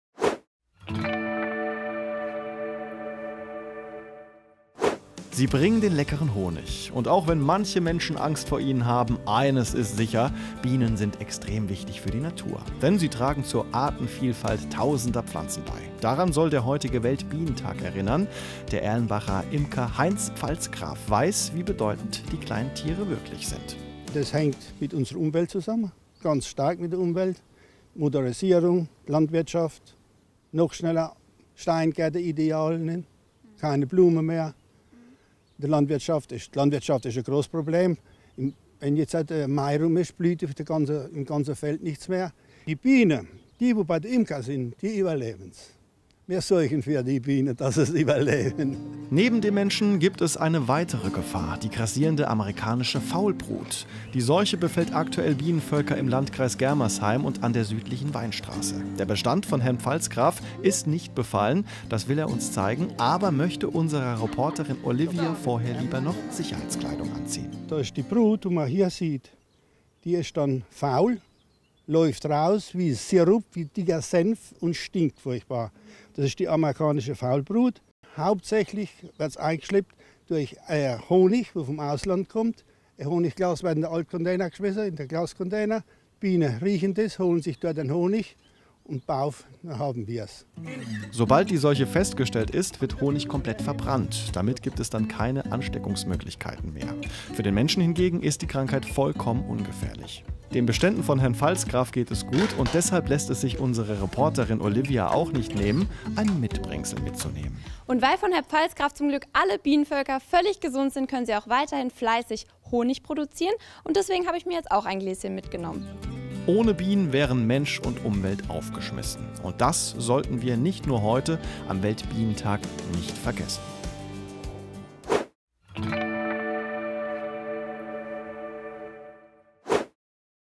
Bienenseuche ausgebrochen | RON TV
Weltbienentag: Heute ist Weltbienentag, doch die Tiere haben selber kaum etwas zu feiern. Denn bei uns in der Region ist die so genannte "Amerikanische Faulbrut" in Bienenkästen ausgebrochen, genauer gesagt im Kreis Germersheim.